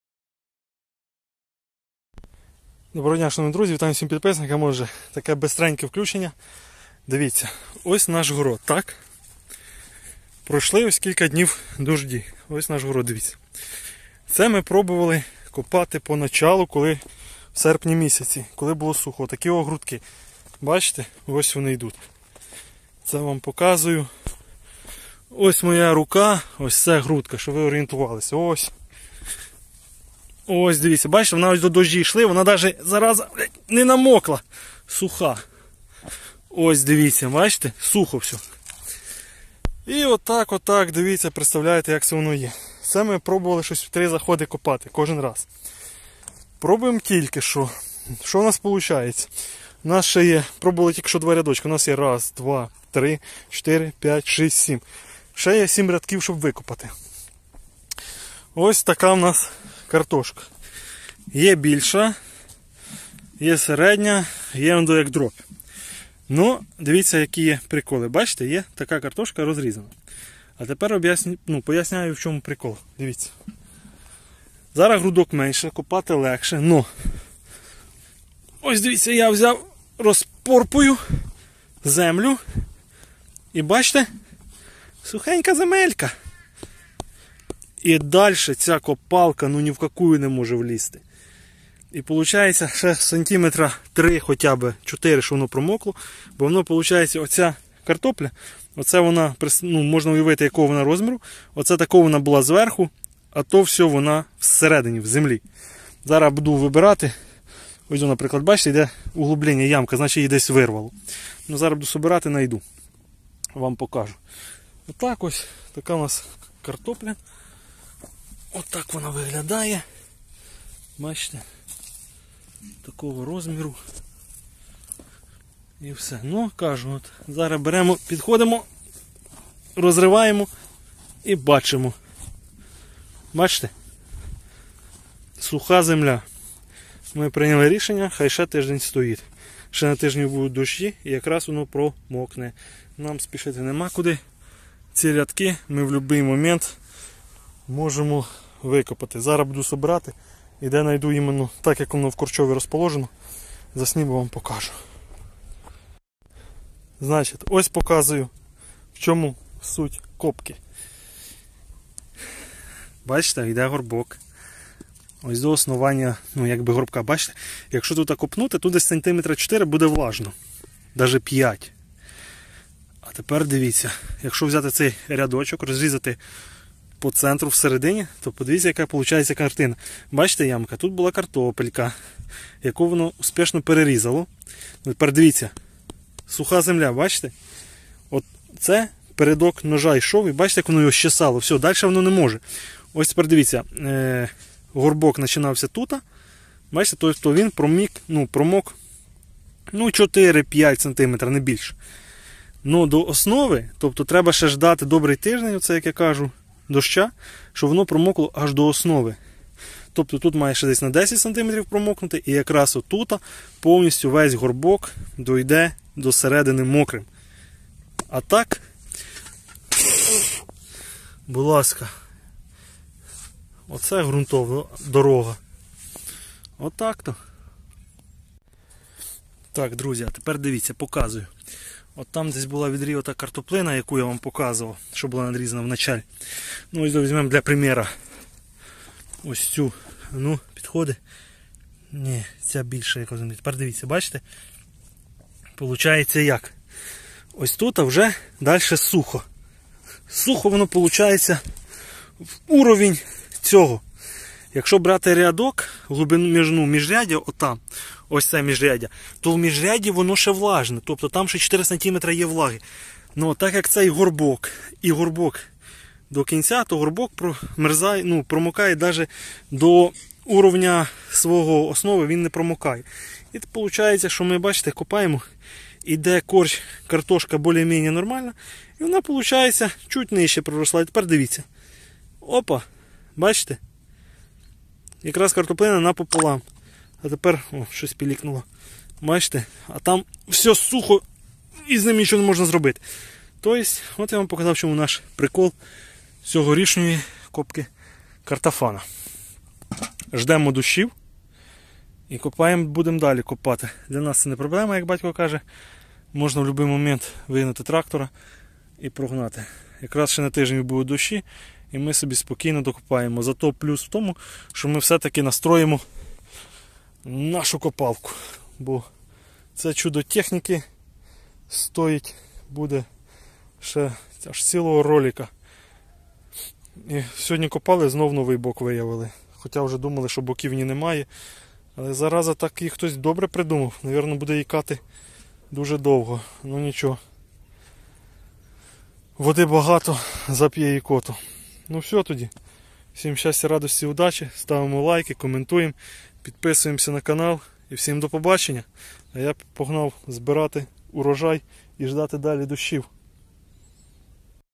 Я буду копать картошку 31 декабря, или Минитрактор шифенг сф-240 не побидим))))
І знову пробуєм копать картоплю ))))